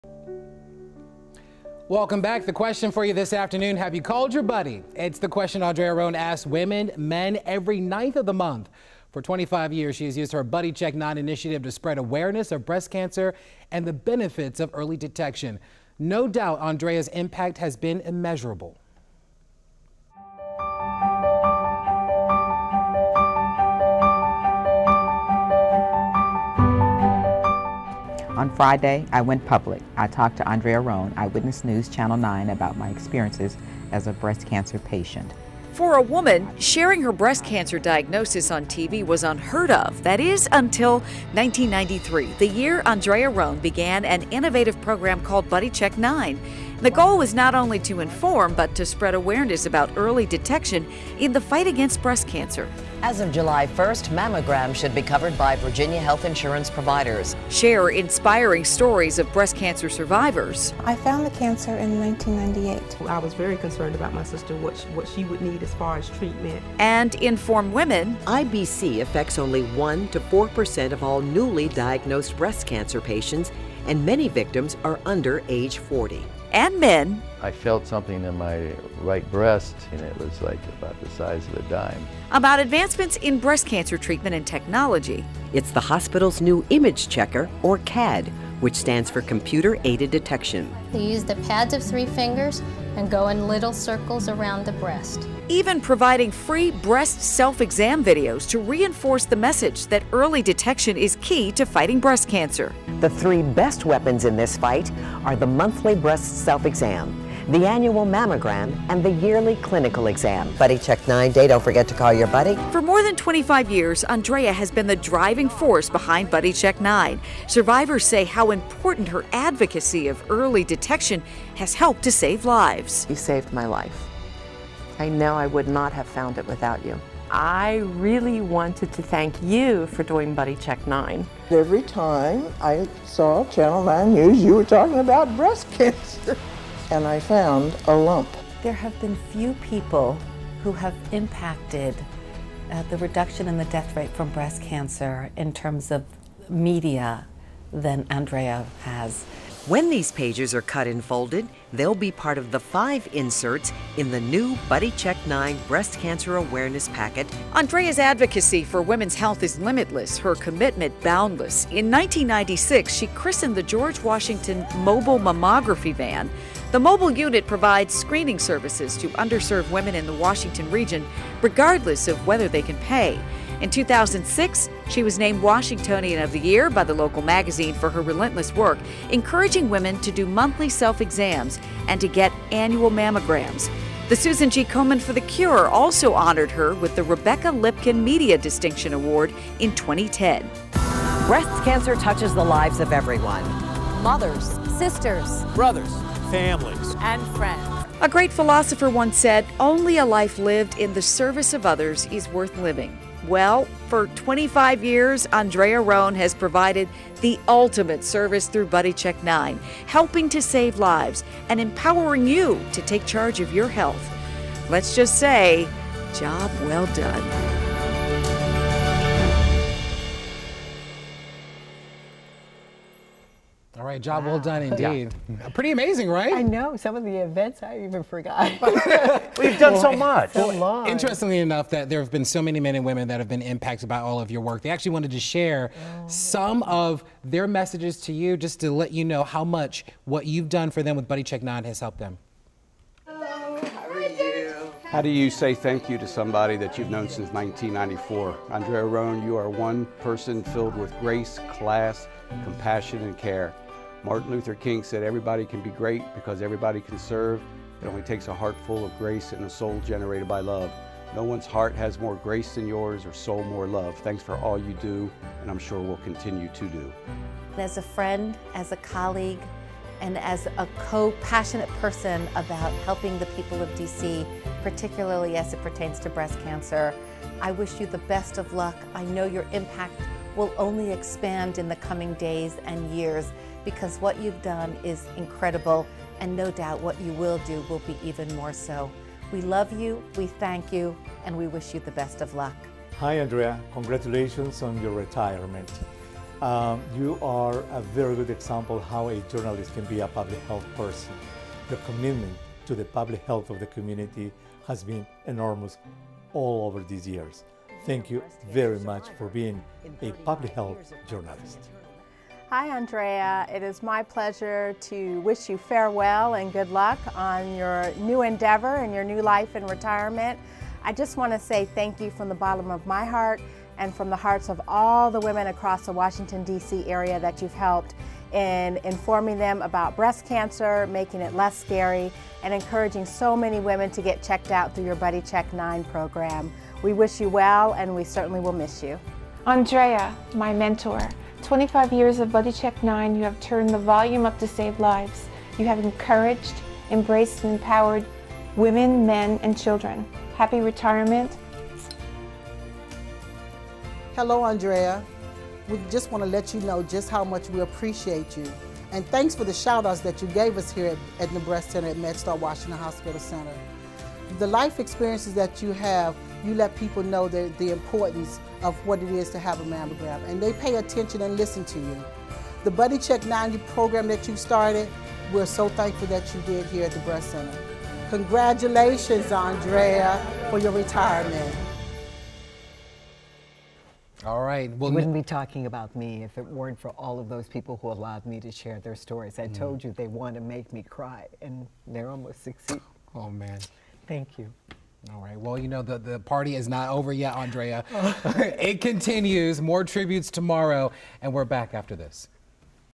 Andrea Roane's 37 years of excellence: Buddy Check 9
"Have you called your buddy?" It's the question Andrea Roane asks women and men every ninth of the month. For 25 years, she has used her Buddy Check 9 initiative to spread awareness of breast cancer and the benefits of early detection. No doubt, Andrea's